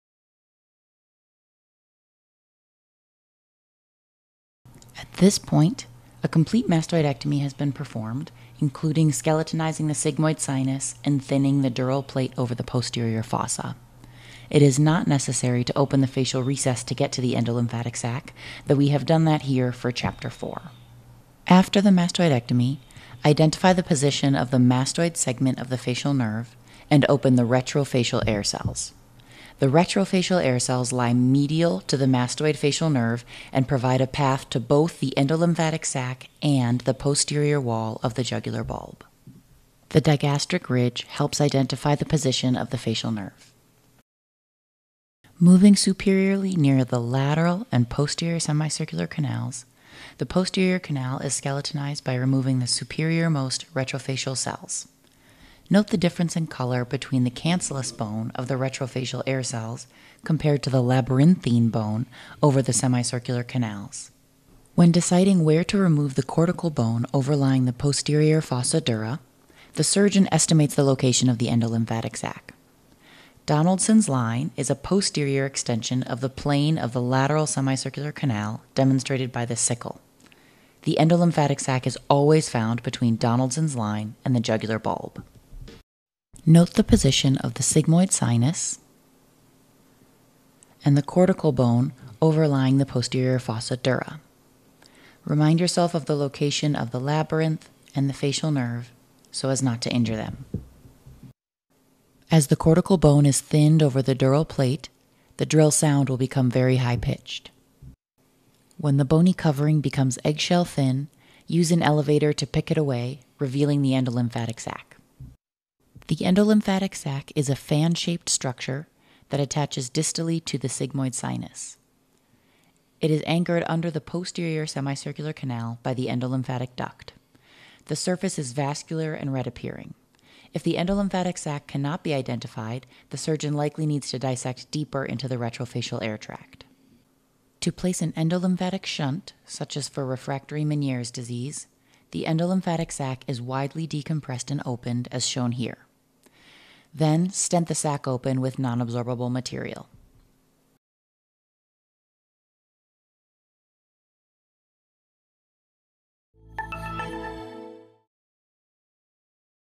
Temporal Bone Dissection: Endolymphatic Sac Decompression (Chapter 5)
This is a continuation of the Mastoid Drilling Techniques Video (Chapters 1-4). Here, we demonstrate finding, exposing, and opening the endolympahtic sac!
As always, still shots of anatomy can be found at the Google Drive